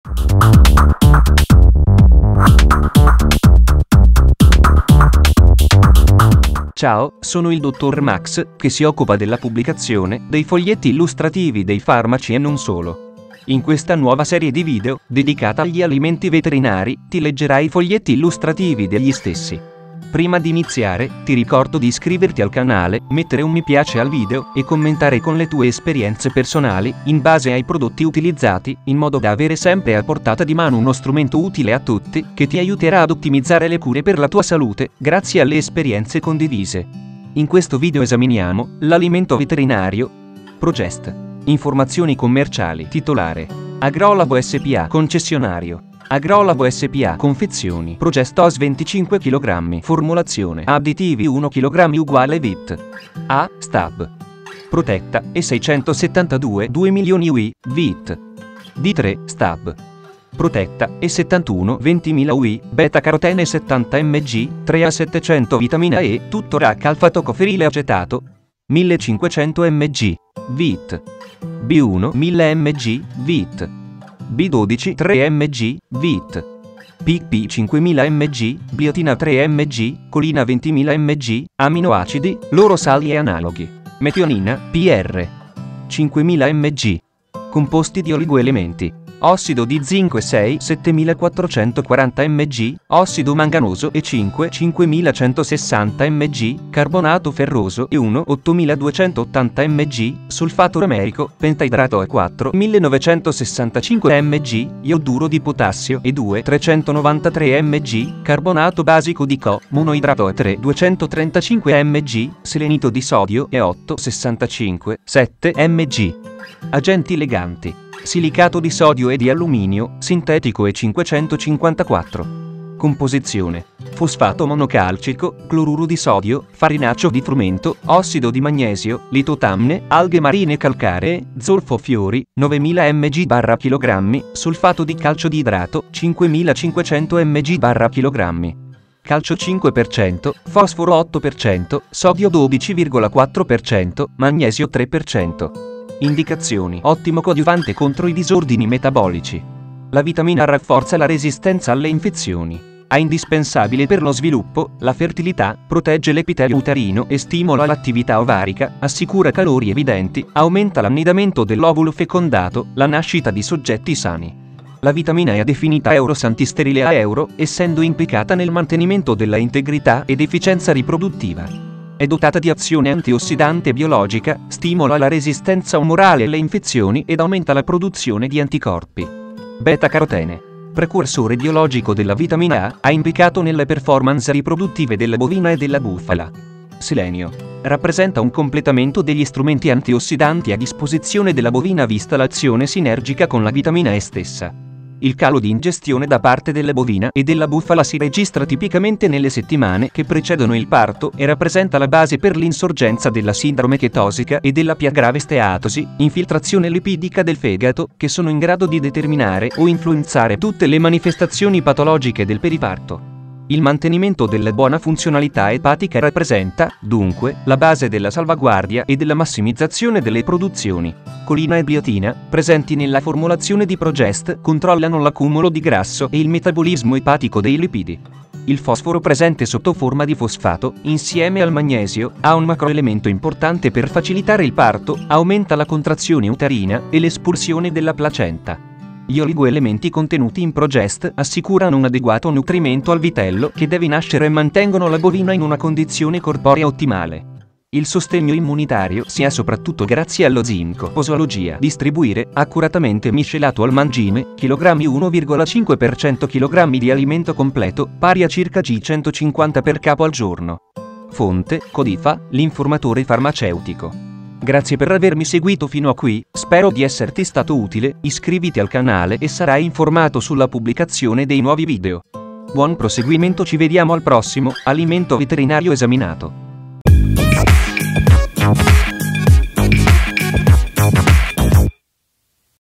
🍪 PROGEST Alimento Veterinario a Cosa Serve ? 🩺 Info Salute Salute
❤️ 🩺 Info Salute: Abbonati a questo canale per accedere ai vantaggi

Abbiamo letto il Foglietto Illustrativo e parlato dell' Alimento Veterinario: PROGEST

Capitoli ⏱
0:00 Introduzione
0:06 Presentazione
0:40 Alimento Veterinario PROGEST
1:01 A cosa serve ?

PROGEST Alimento Veterinario